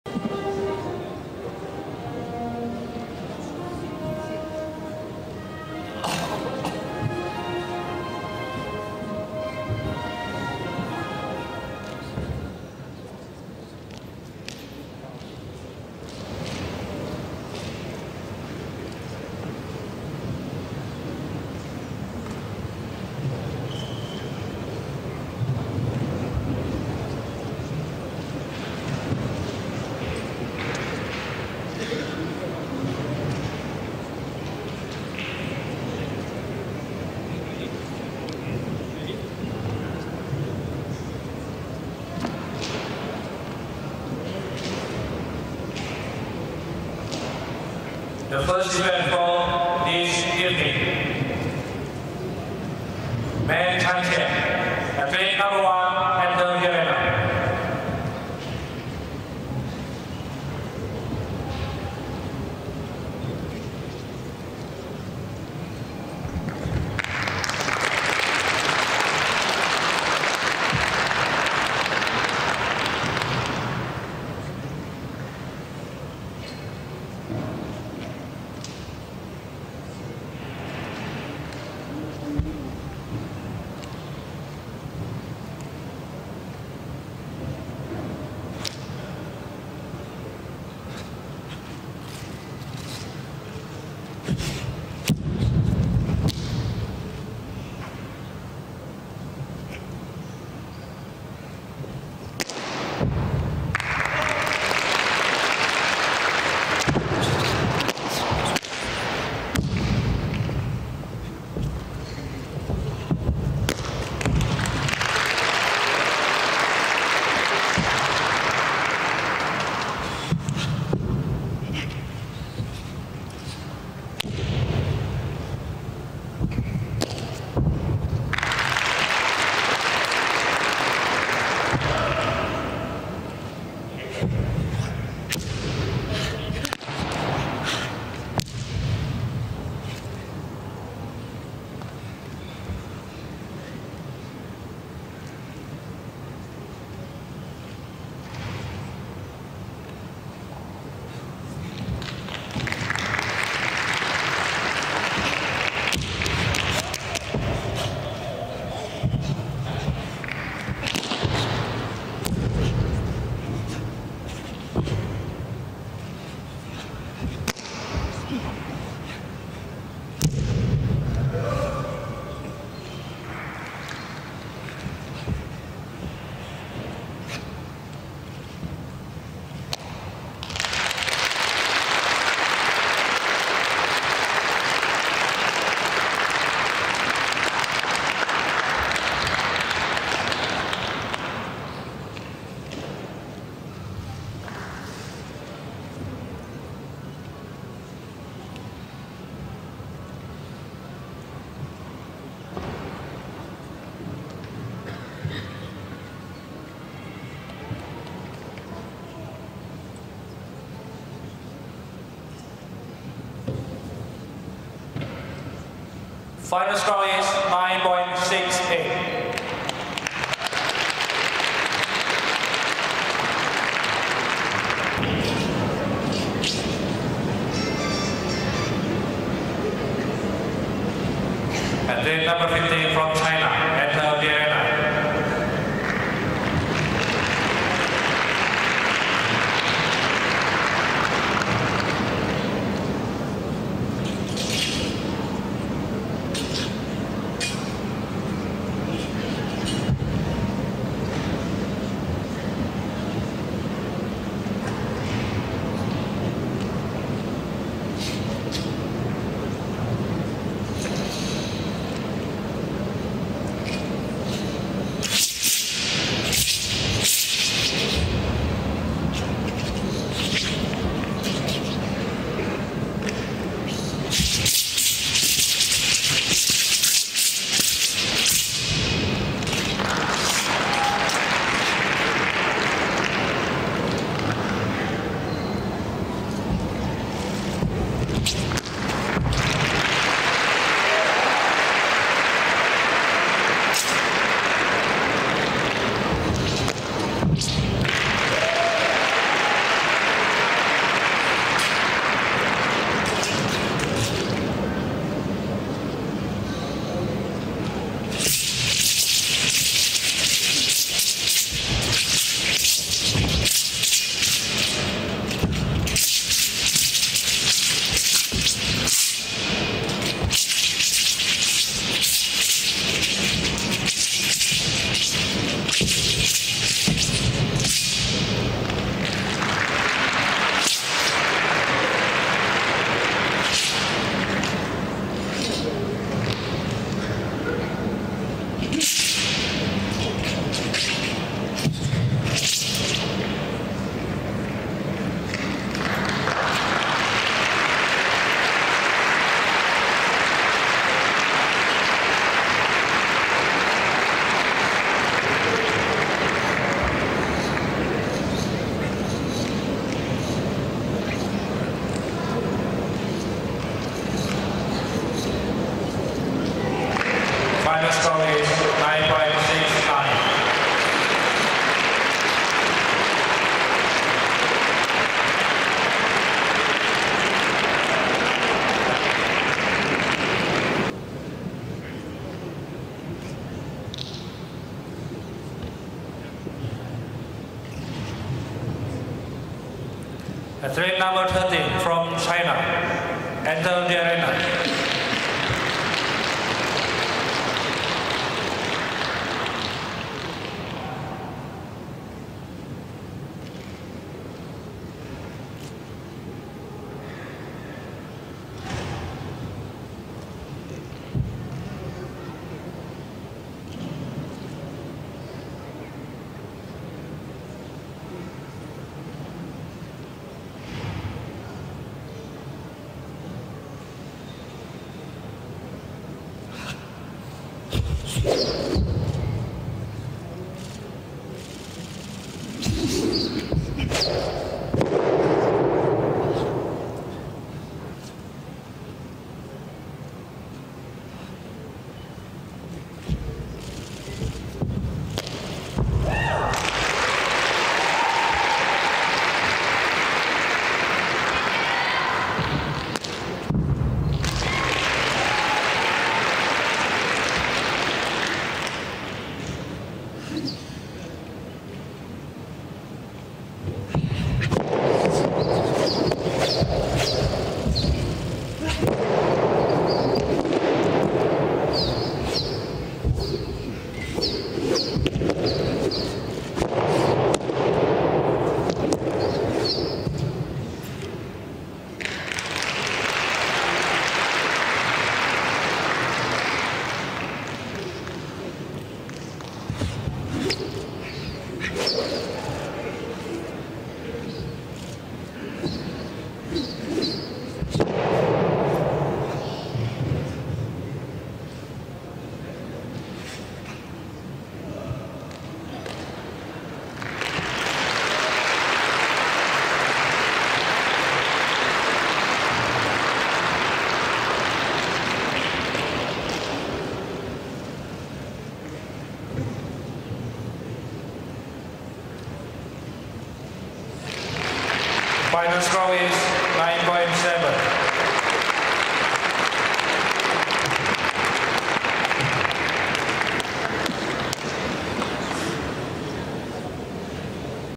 2nd SportAccord World Combat Games (2013) - Wushu (Taolu) - Men's CQ, DS, GS -1st Place
Lei YU (CHN) - 1st Place Men's Changquan (Long Fist), Daoshu (Broadsword) & Gunshu (Cudgel） Combined - Taolu Video